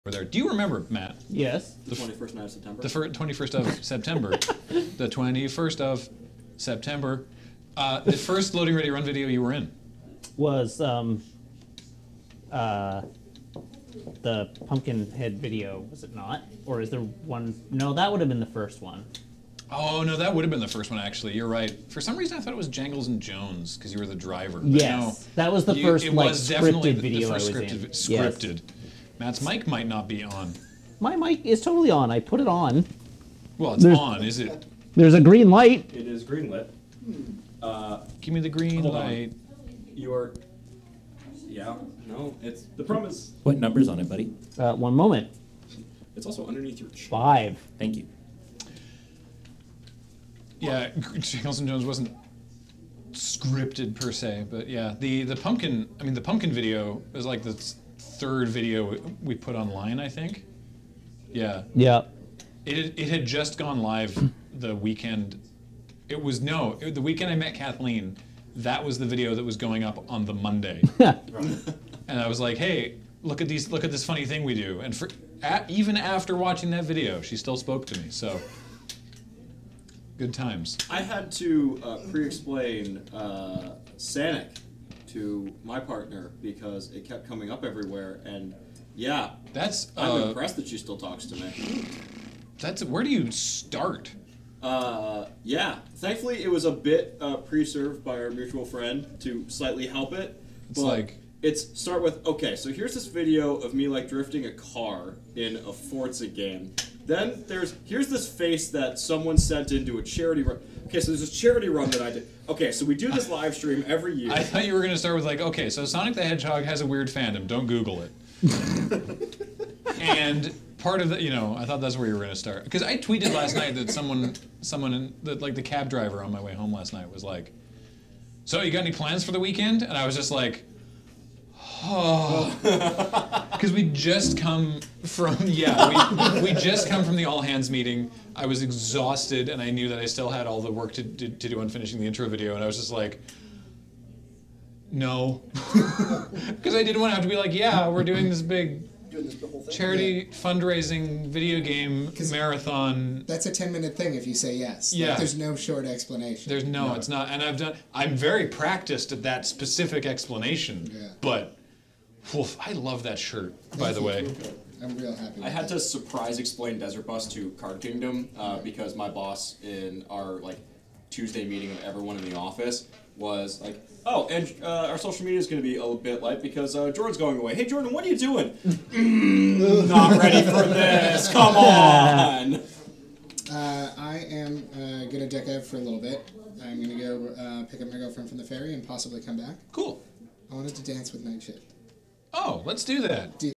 DB2019 - Matt's first LRR video and Jordan had to explain Sanic
Matt's first LRR video and Jordan had to explain Sanic to his SO